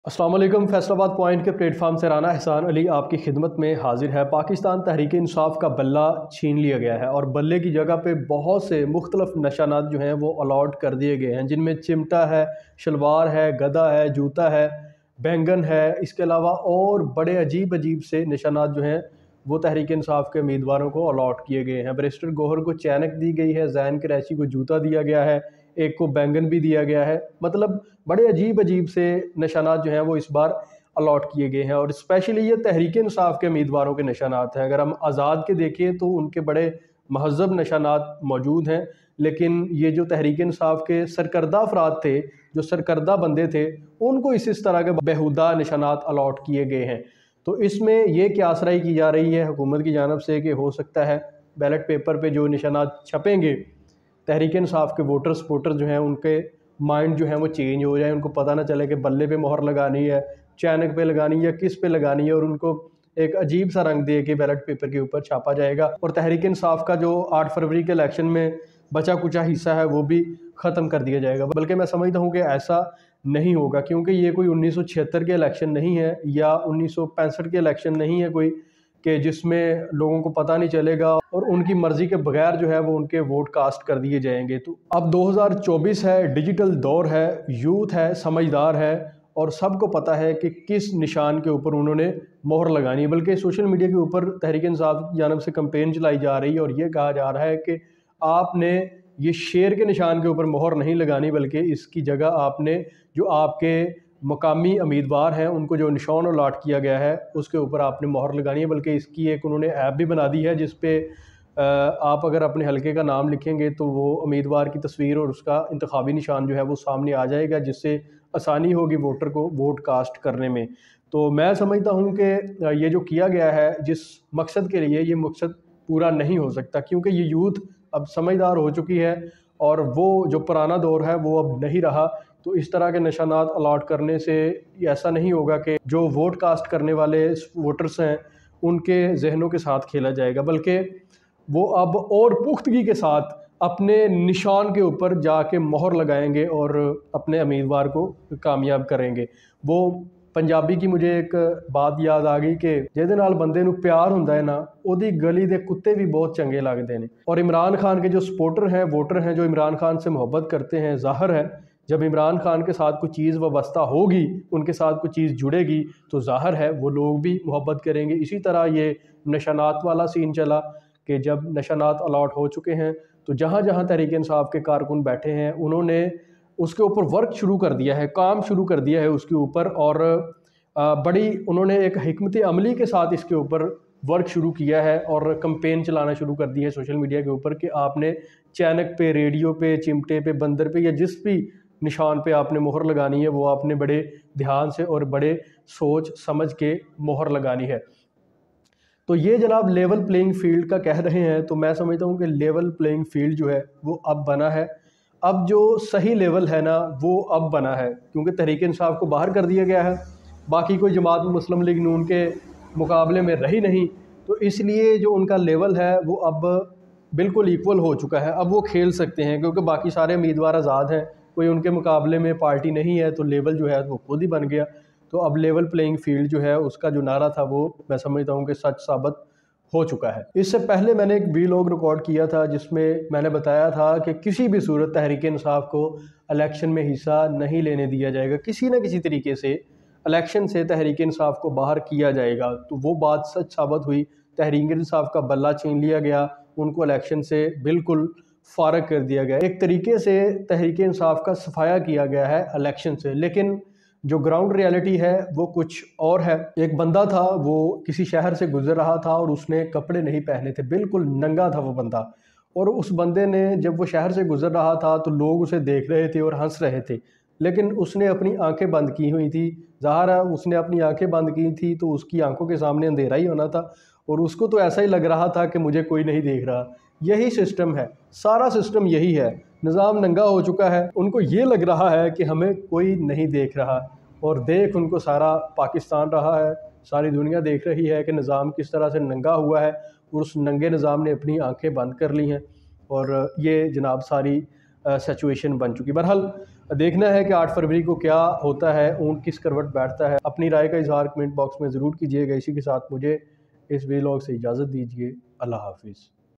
چمٹا جوتا بینگن شلوار گدا تحریک انصاف کو عجیب عجیب نشان الاٹ
ایسے انتخابی نشان کیوں الاٹ ہوئے عمران خان غصے میں آگئے نئی حکمت عملی پر کام شروع